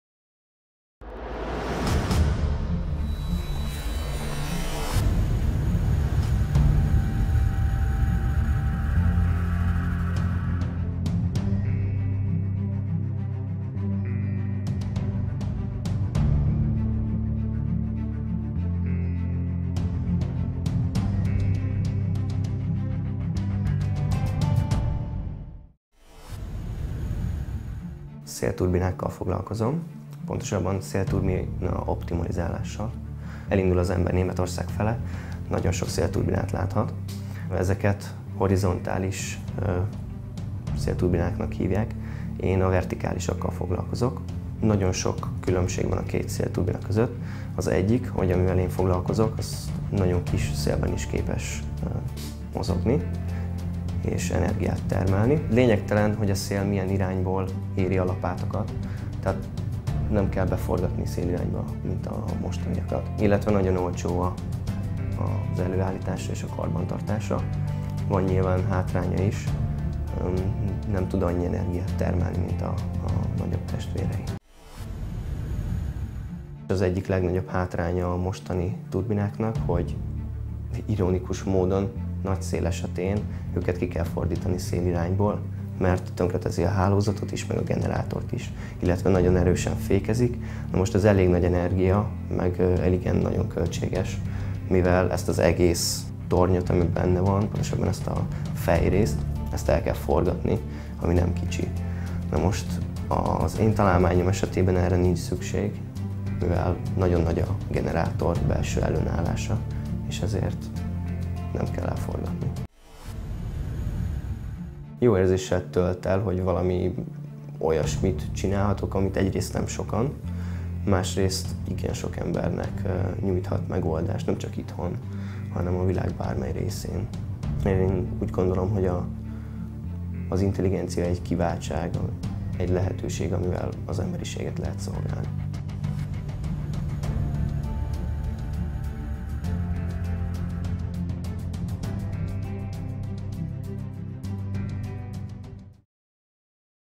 Kutatói portrék - Fröhlich Balázs: X turbina
A Pécsi Tudományegyetem kutatói portrésorozatot indít az itt folyó tudományos munka eredményeinek, sokszínűségének bemutatására. Induló projektünk célja, hogy bemutassuk az egyetem vonzáskörzetében található fiatal korosztály számára ezen munka értékeit, eredményeit, ezáltal ösztönözve hallgatóinkat saját kutatói tevékenység folytatására.

A PTE Universitas Televízió által készített portrésorozat következő részében Fröhlich Balázs, a PTE - PMMIK hallgatója mutatja be kutatásának eredményeit.